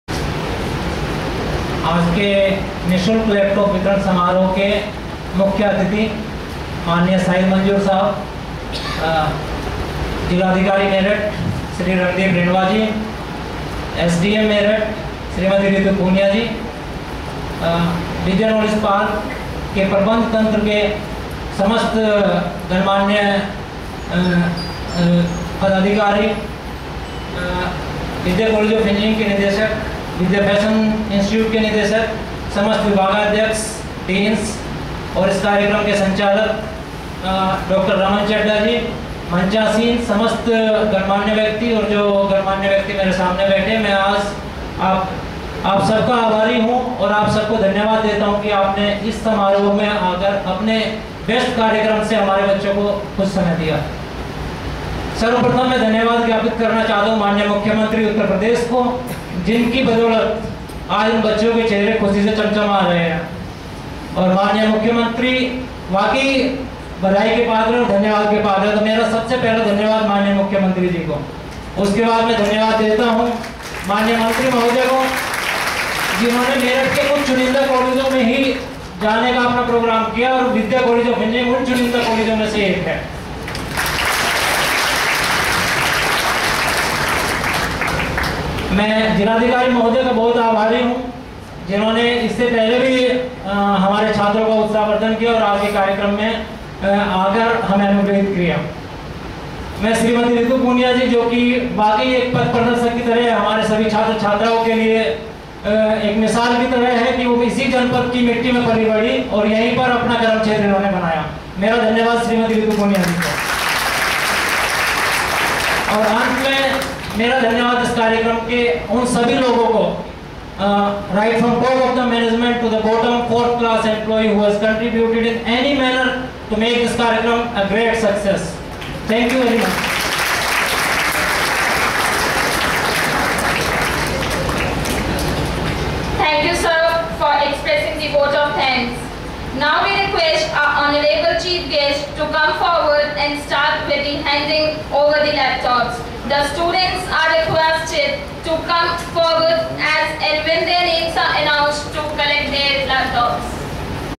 Laptop Distribution Program at Vidya Knowledge Park Meerut Part II
We are very much thankful to Mr.Shahid Manzoor (Honorable Minister, Labor & Employment, Government of Uttar Pradesh) , Mr.Navdeep Rinwa (District Magistrate, IAS, Meerut) & Ms. Ritu (SDM) for glorious presence in Laptop Distribution Program by Government of Uttar Pradesh at Vidya Knowledge Park Meerut